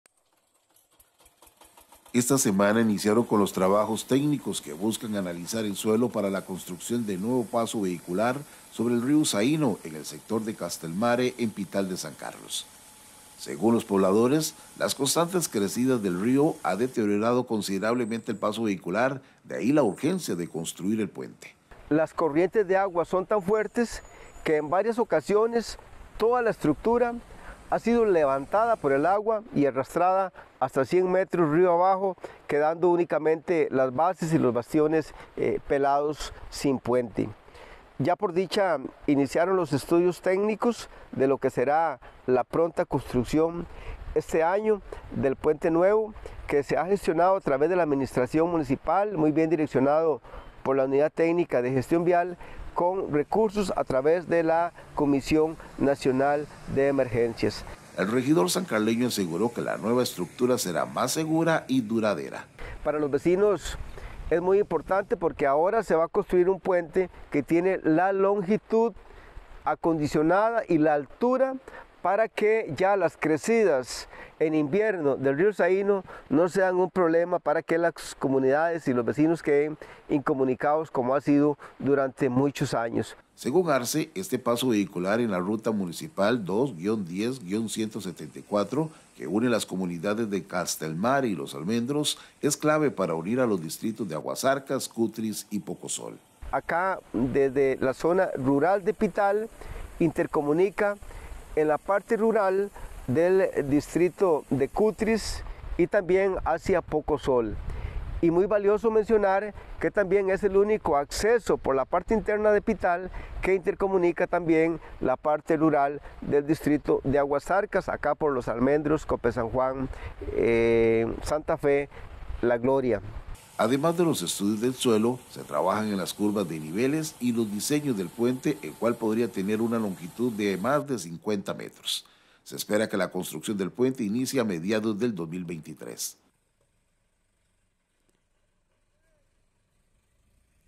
PUENTE SOBRE EL RÍO SAÍNO EN PITAL DE SAN CARLOS SERÁ RECONSTRUIDO
Los estudios técnicos iniciaron esta semana.